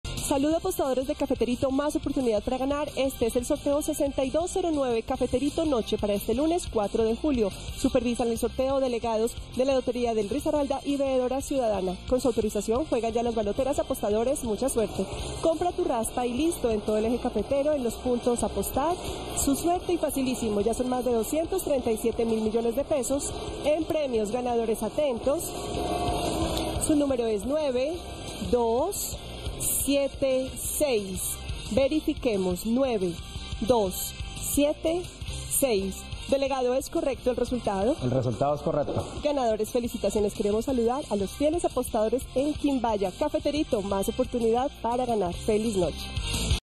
CAFETERITO NOCHE Resultado del LUNES 04 DE JULIO de 2022 ✅🥇🔥💰
Hola buenas noches, te presentamos los resultados de las loterías y chances en Colombia, vamos con el Resultados del Chance El Cafeterito noche del LUNES 04 DE JULIO de 2022.